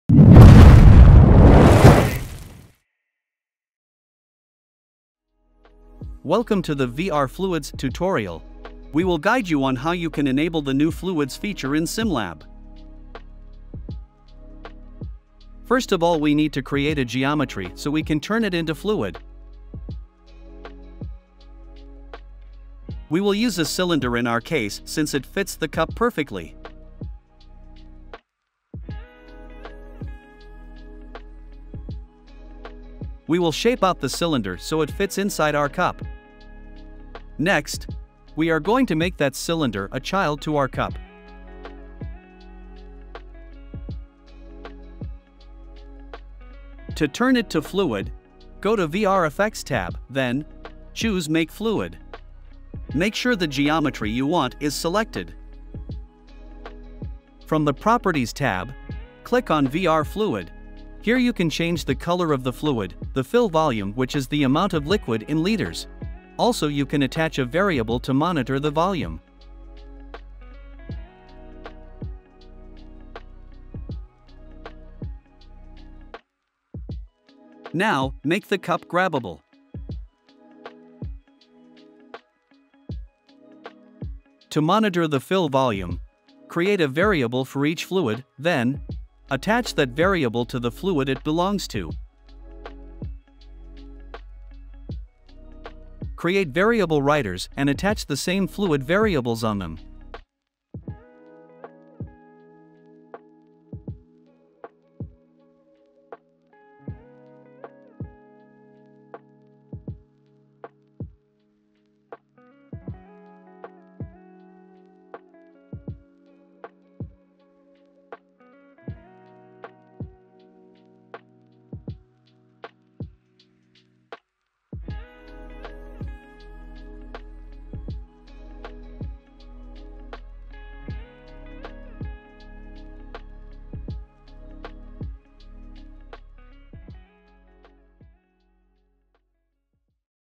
Using the Fluid Effect in VR
This tutorial will guide you through using the Fluid effect in Virtual Reality (VR). You'll learn how to:

View fluid in a VR environment.
Pour fluid from one container to another.
Track the amount of fluid in each container.
The VR Fluid feature is included in SimLab Composer.